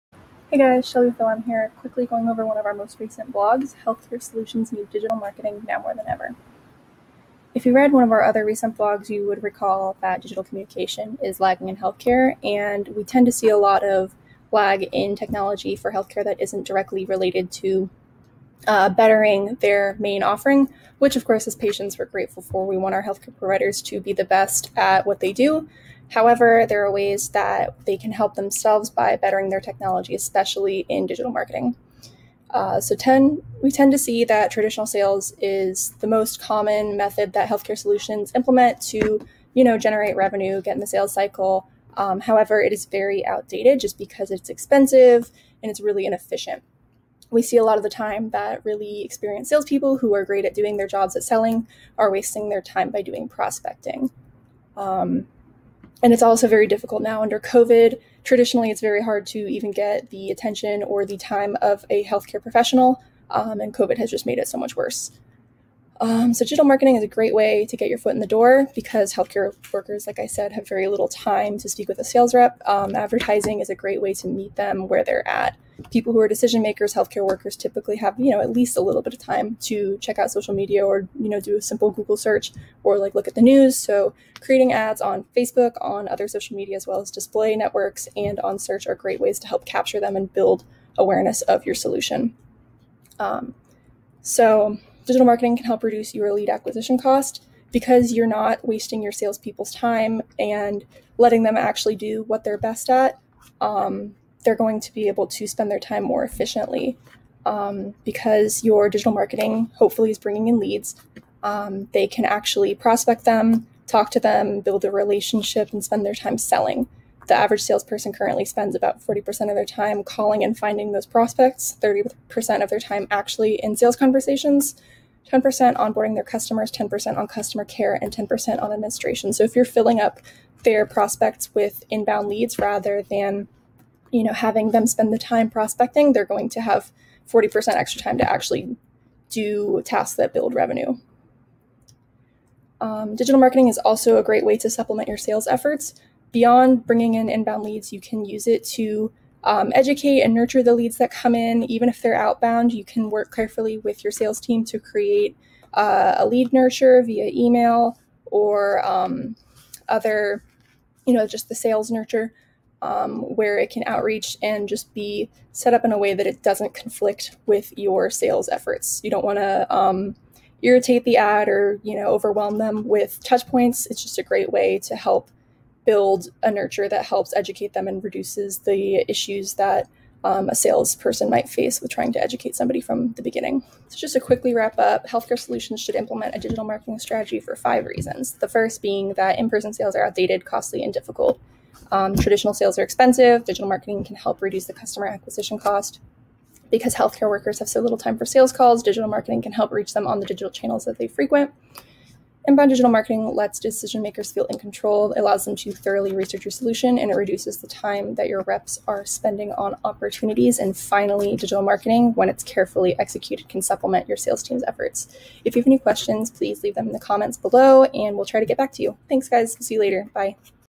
Healthcare Solutions Need Digital Marketing Now More Than Ever
Traditional healthcare sales has been outdated for years. Inbound digital marketing is a growing, sustainable strategy to allow healthcare solutions to scale. By implementing an inbound digital marketing strategy, salespeople can focus on selling the solution instead of spending their time less efficiently by prospecting.